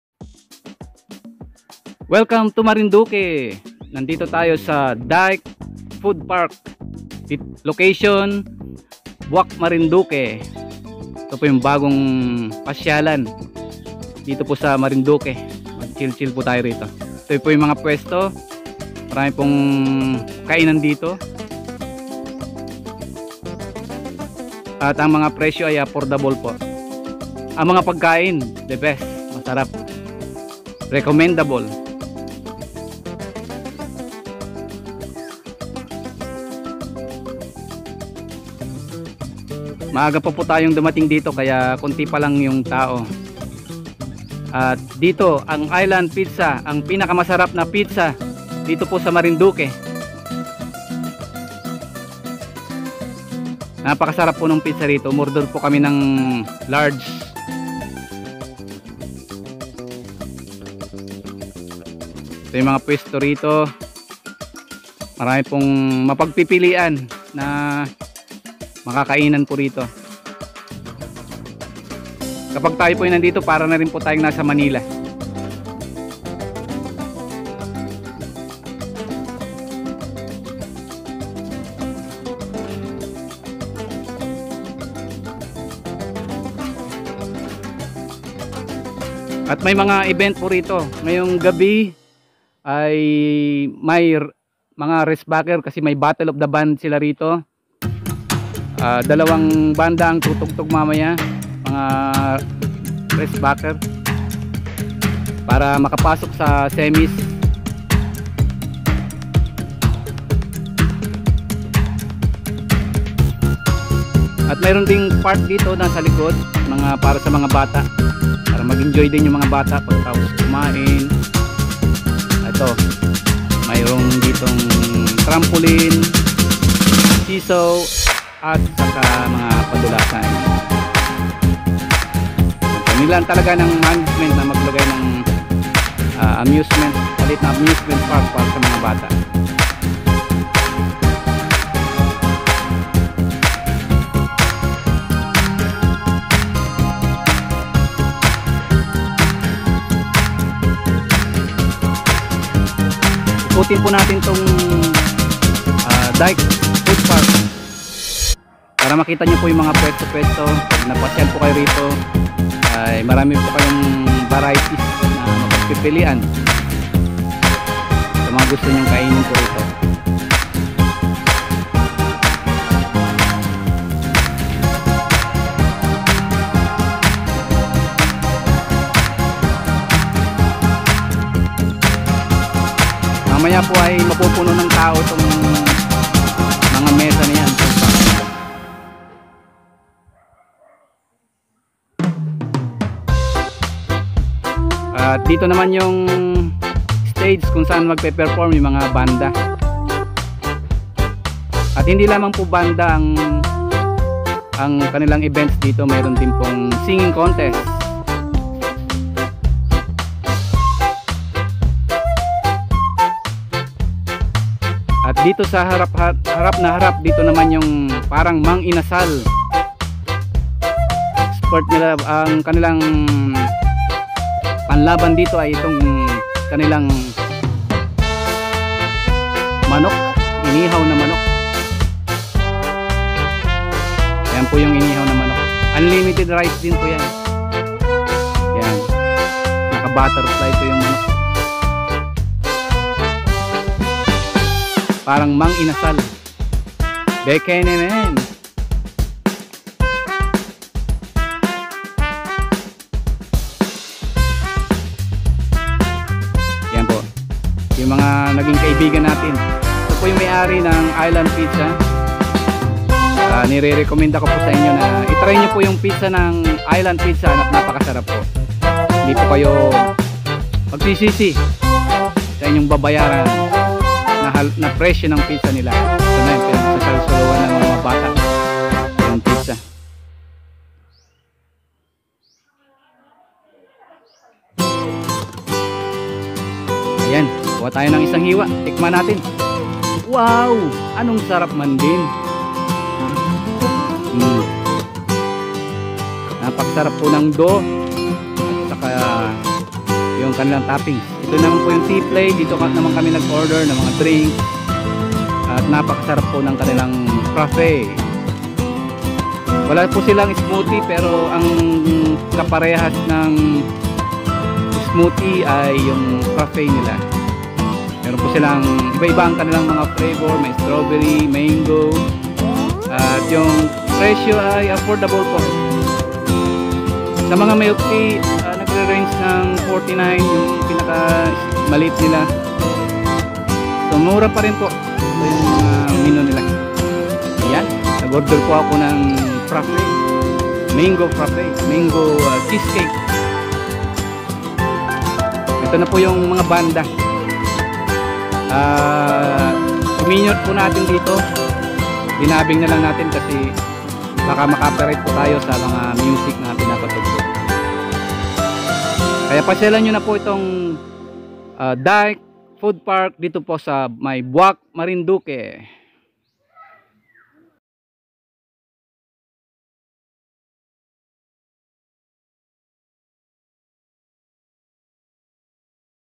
DYKE FOOD PARK Boac Marinduque Philippines 🇵🇭 Bagong Pasyalan at may Live Band
Mga Adventure ni Pilkan Chief dito sa Pilipinas